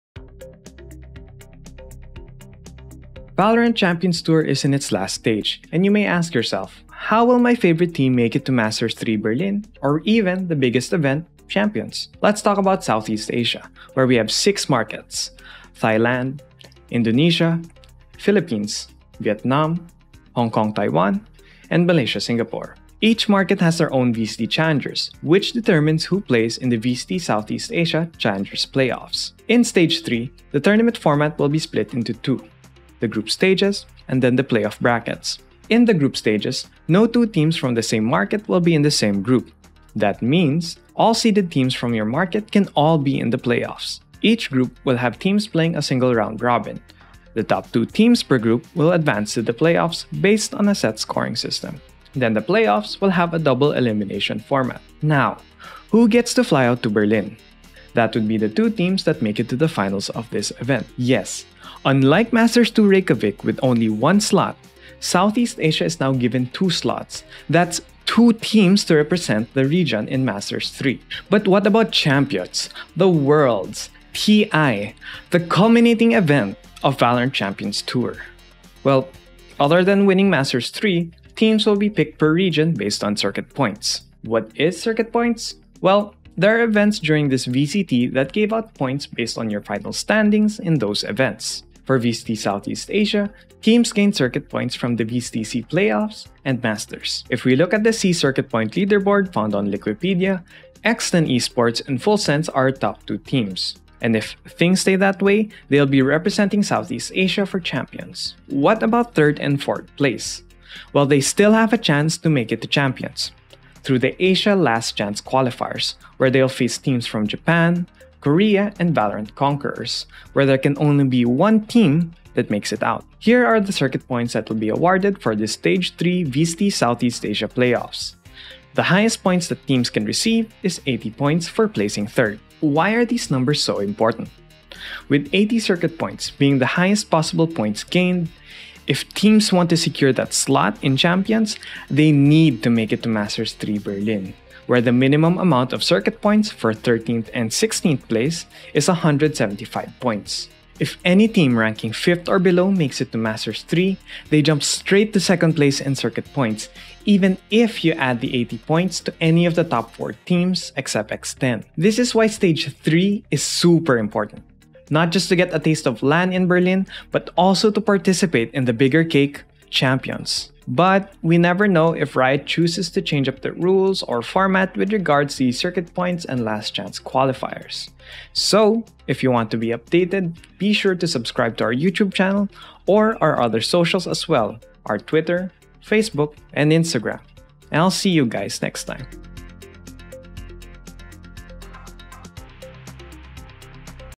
SEA VCT Stage 3 - Circuit Points Explanation | Post-Plant Chat
VALORANT Champions is just around the corner! The SEA Playoffs will determine which two teams will represent SEA and fly over to Berlin. Other than winning Masters 3, teams will be picked for the Champions based on Circuit Points.

Hosted by: TheJoaquiGamer
Head Editor: amiwa
Assistant Editor: Rafii
Thumbnail by: Lourd